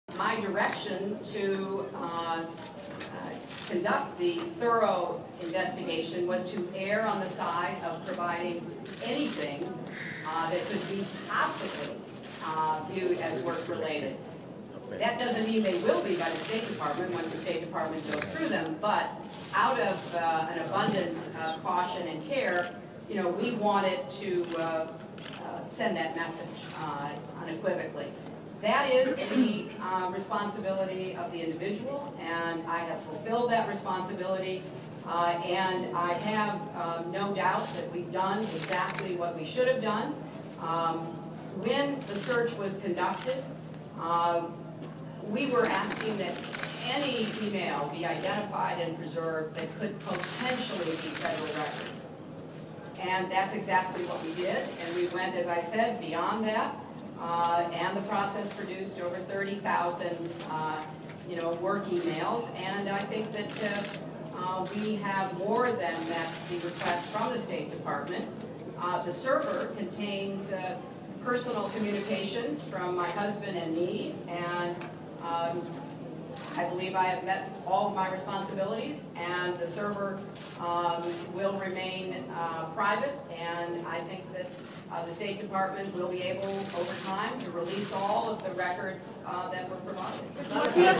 Hillary Clinton s'explique sur sa messagerie privée
Conférence de presse à l'ONU le 10 mars 2015. L'ex-secrétaire d'Etat répond à la controverse sur sa messagerie privée. Quand elle dirigeait le Département d'Etat, elle n'a jamais utilisé la messagerie officielle de l'administration